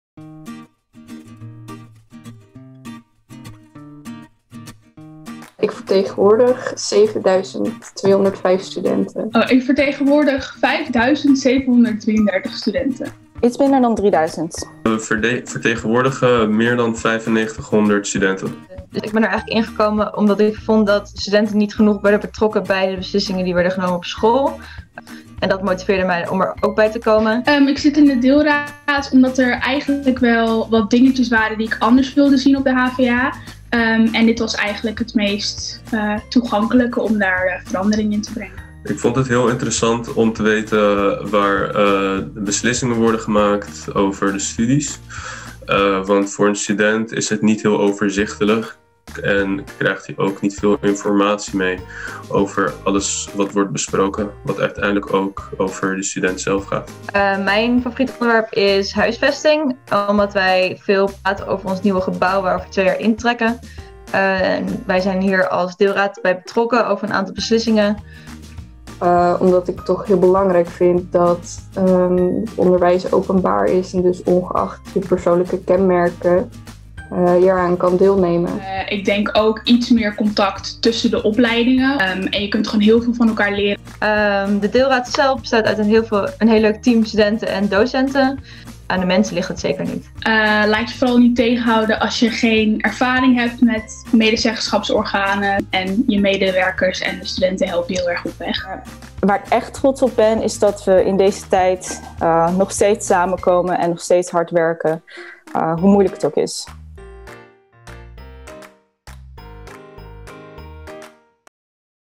Stel je kandidaat. Juist nu! - medezeggenschap HvA
HvA-studenten van verschillende medezeggenschapsraden vertellen wat het inhoudt en waarom ze lid zijn geworden, wat ze doen, wat ze bereikt hebben en waarom het zo leuk is - en misschien ook voor jou! Stel je dan kandidaat. Juist nu!

Ga tussen 15 maart en 11 april 2021 naar verkiezingen.mijnhva.nl en stel je kandidaat. Juist nu!